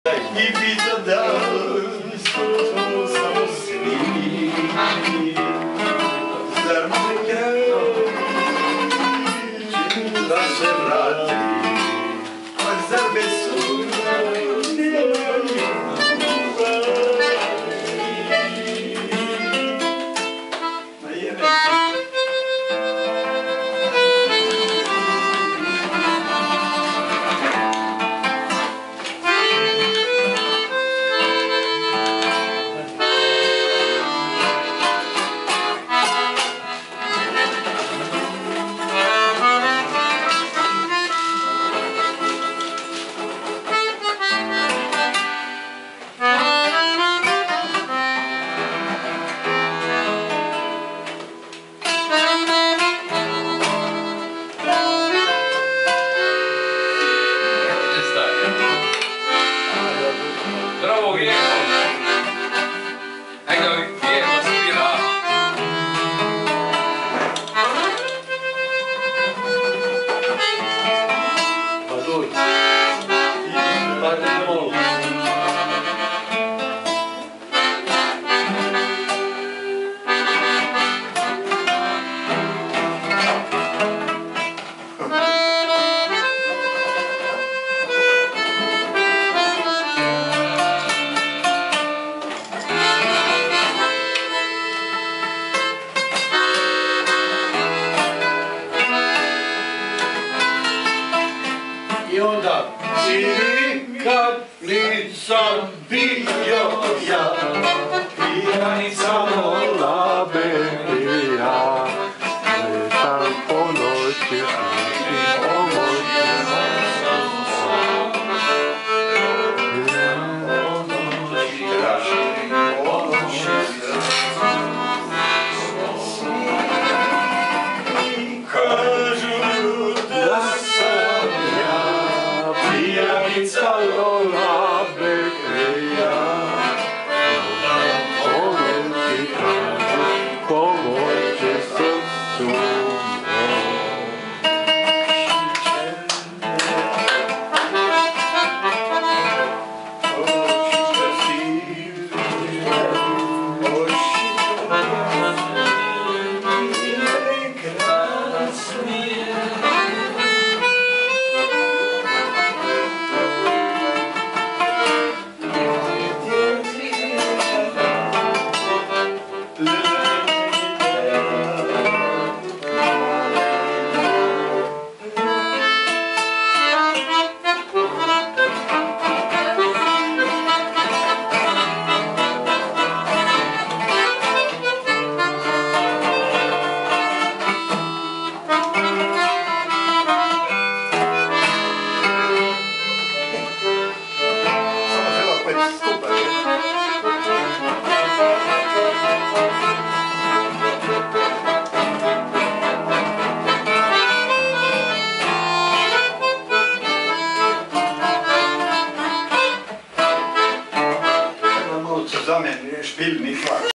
Četiri muzičke legende ovog petka u ateljeu.
Zlatić - Barač - Polić - Perković.